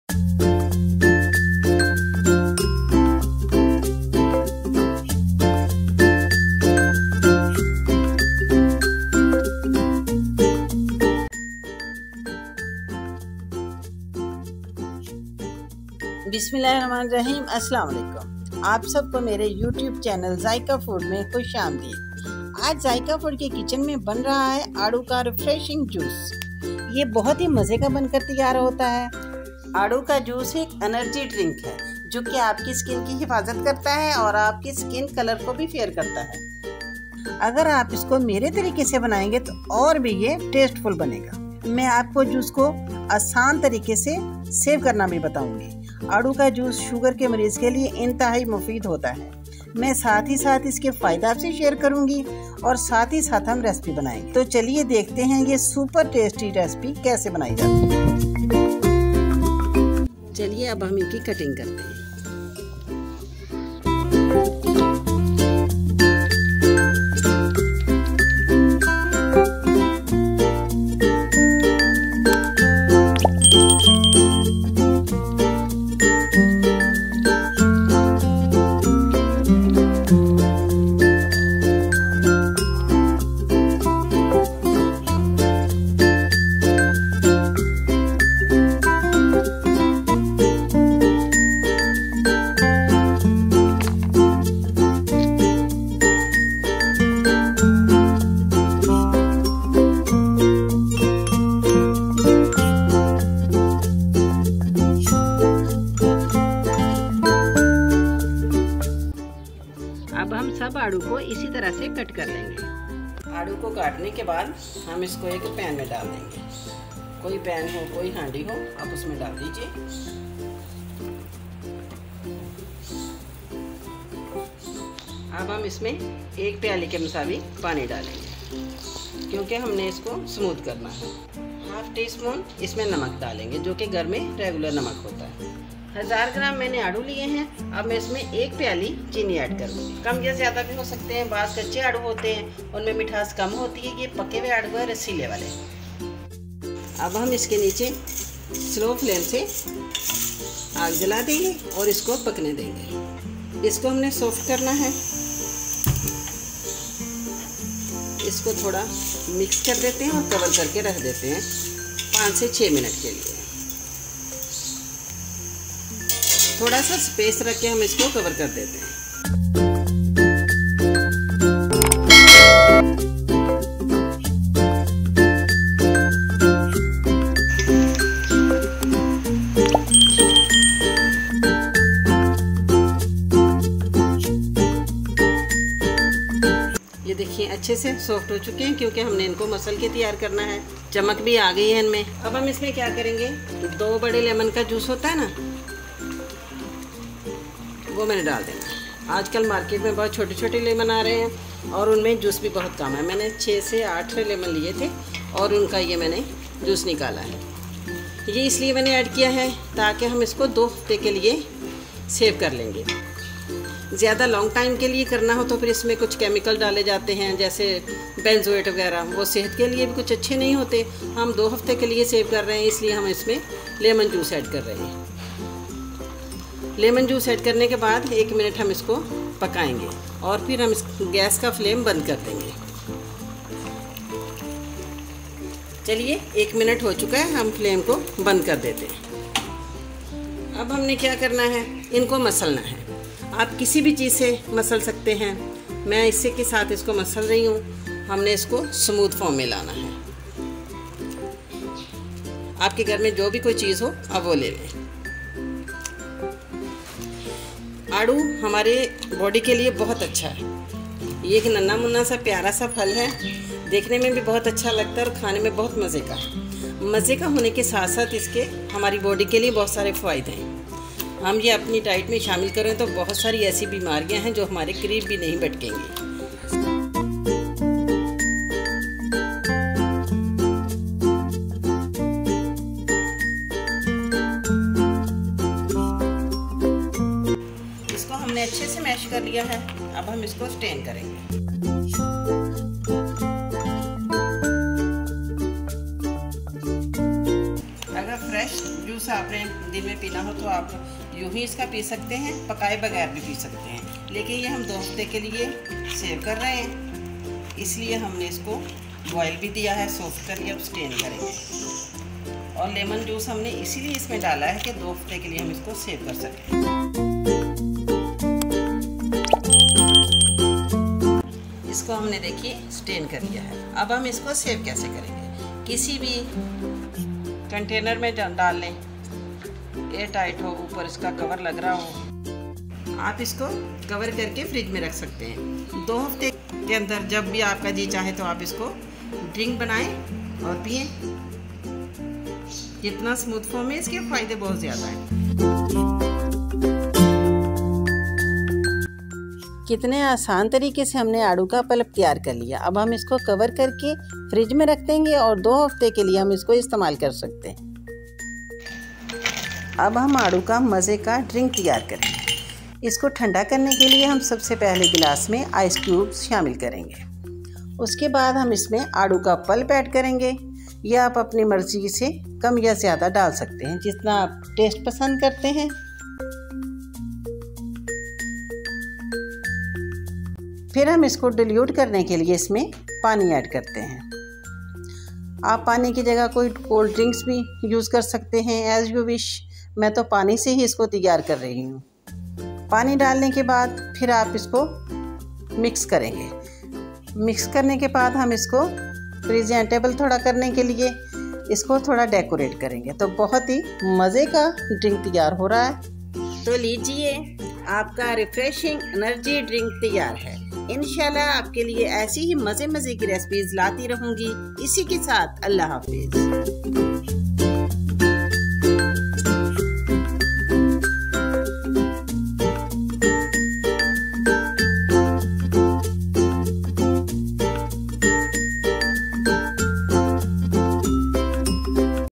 Refreshing peach Juice |آڑو کا شربت | Easy Summer Drinks Recipes| Zaika Food
Assalam o alikum Viewers!
My this video is all about how to make Summer Drinks 😋At Home
Ye bohat Fast Aur Easy recipe ha aap ise buhat easily Ghar pe asani se bna sakte han
Agar aap ko Meri recipe ka idea acha Laga to Meri recipe ko like aur share kren

Her is my Instagram account link

00:00 - Intro
00:59 - Cutting
01:53 - Boiling
04:02 - Meshing
04:48 - Straining
05:24 - Saving
05:59 - Serving
07:12 - End